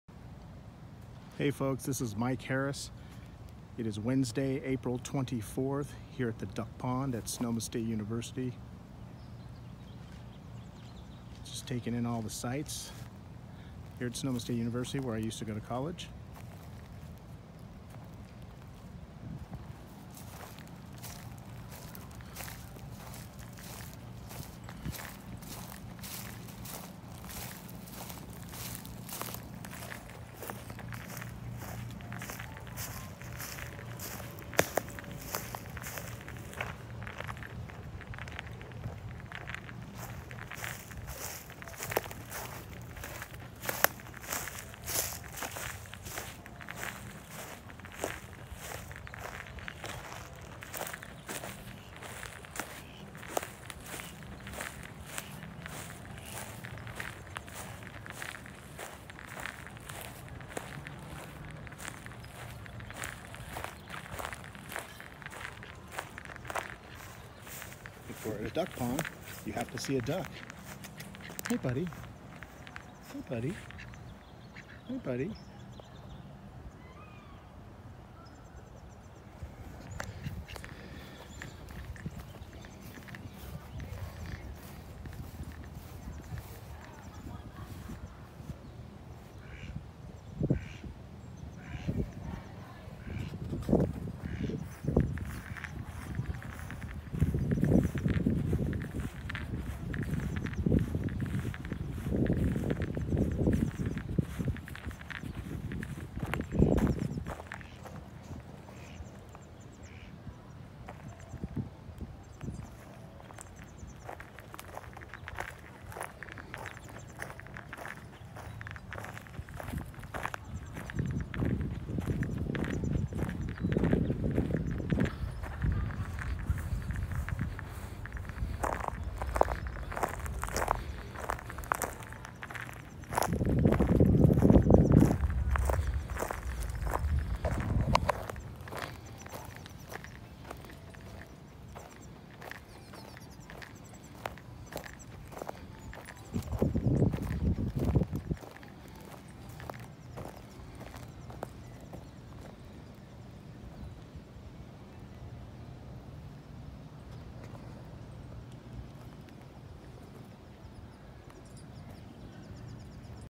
Duck pond at Sonoma State University
A stroll around the duck pond at Sonoma State University in Rohnert Park, California.

Filmed on location by Mike Harris on Wednesday April 24th, 2019.